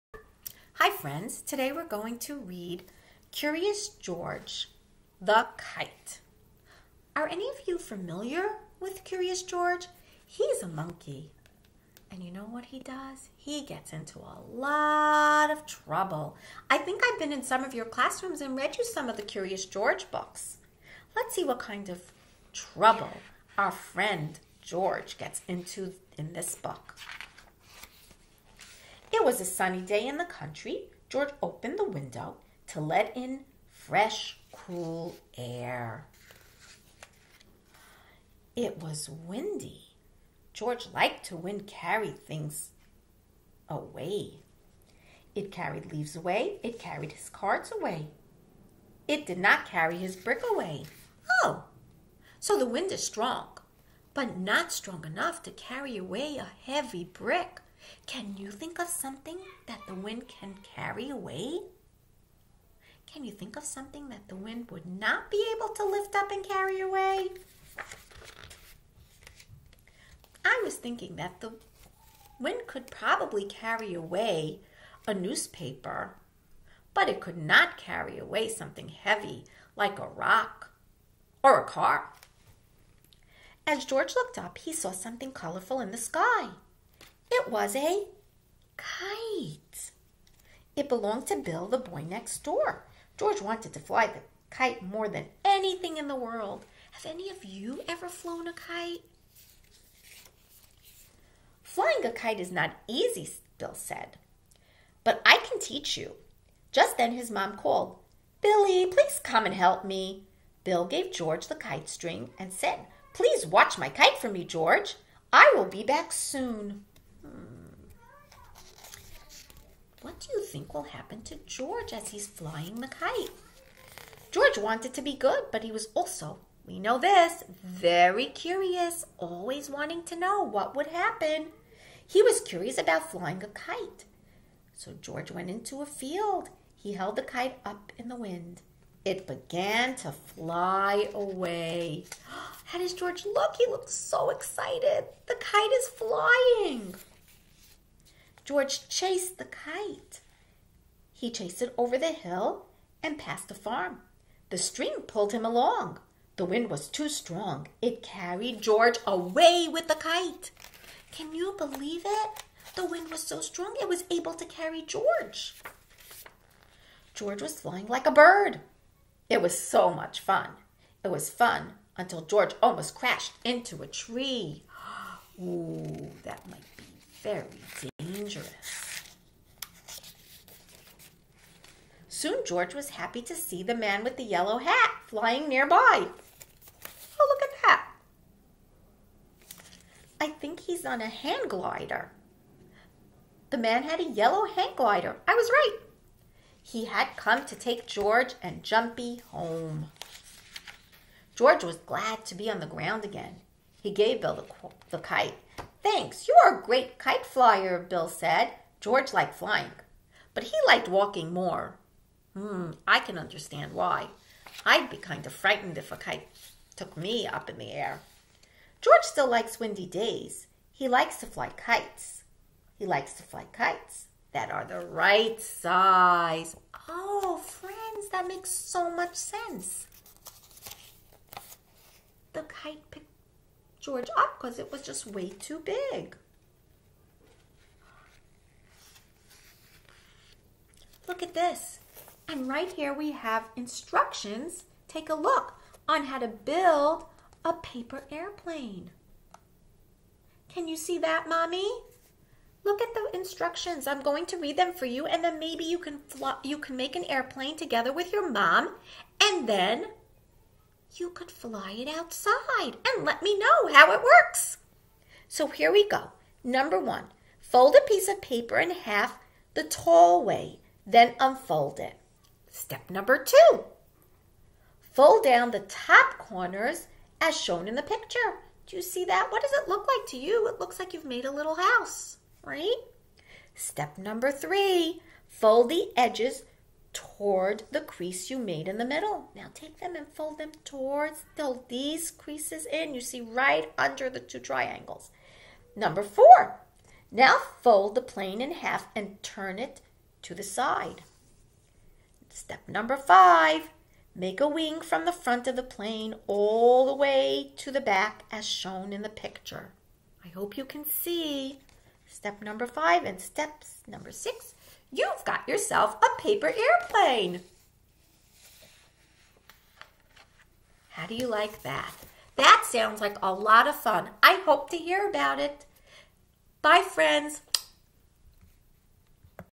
Curious George: The Kite
Morah Carrie reads Curious George